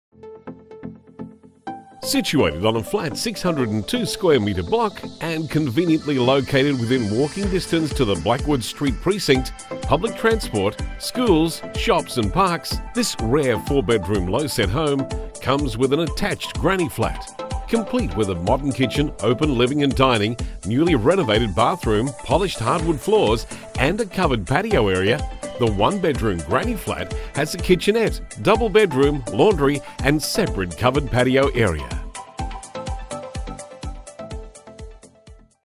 34 Glen Holm Street - Mitchelton (4053) Queensland by Katrina Gibbons
Property Video shoot of 34 Glen Holm Street - Mitchelton (4053) Queensland by PlatinumHD for